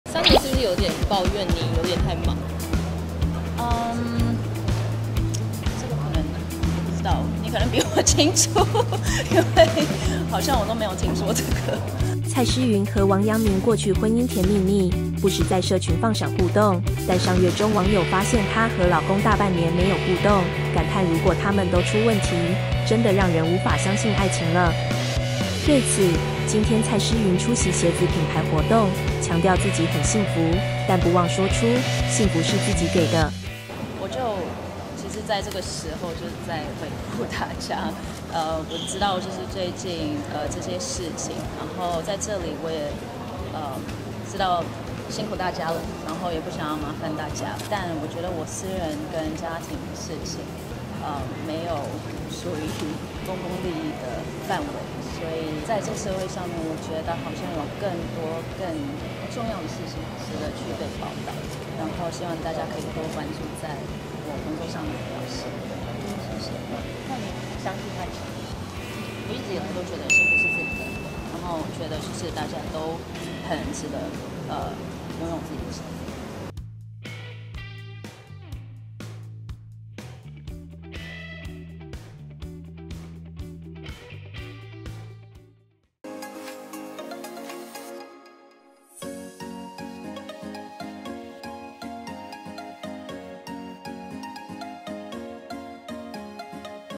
現身了！蔡詩芸爆出和王陽明婚變後首露面 強調：幸福是自己給的
蔡詩芸 和#王陽明 過去婚姻甜蜜蜜，不時在社群放閃互動，但上月中網友發現她和老公大半年沒有互動，感嘆如果他們都出問題，真的讓人無法相信愛情了，對此，今（19）蔡詩芸出席鞋子品牌活動，強調自己很幸福，但不忘說出：「幸福是自己給的！」